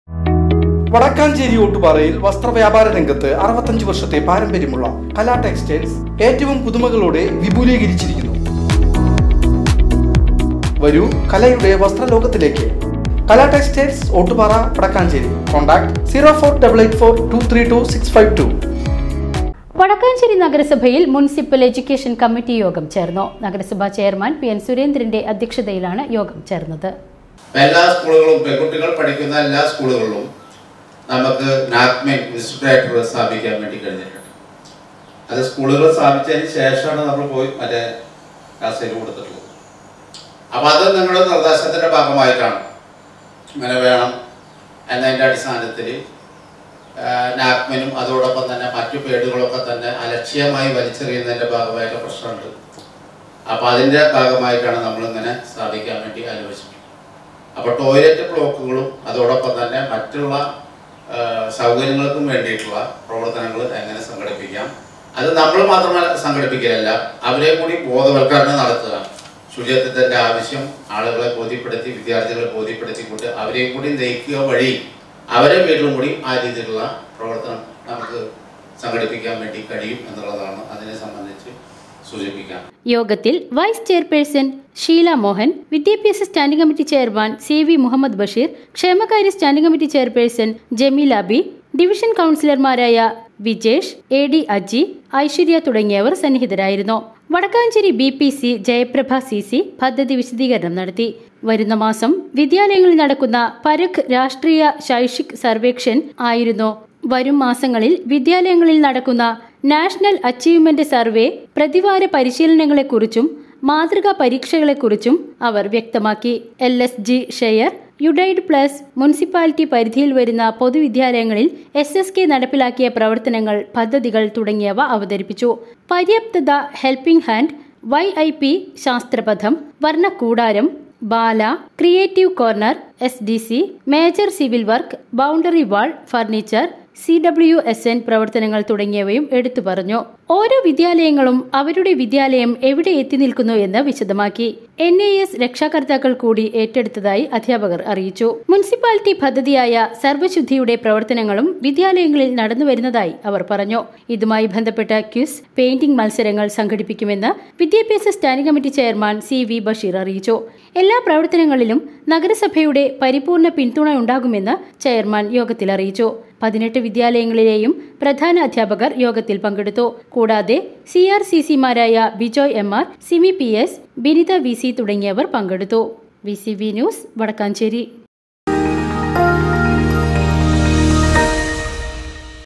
വടക്കാഞ്ചേരി നഗരസഭ ചെയർമാന്റെ അധ്യക്ഷതയിൽ മുനിസിപ്പൽ എഡ്യൂക്കേഷൻ കമ്മിറ്റി യോഗം ചേർന്നു
VCV NEWS

VCV NEWS CHANNEL
Wadakkanchery, Thrissur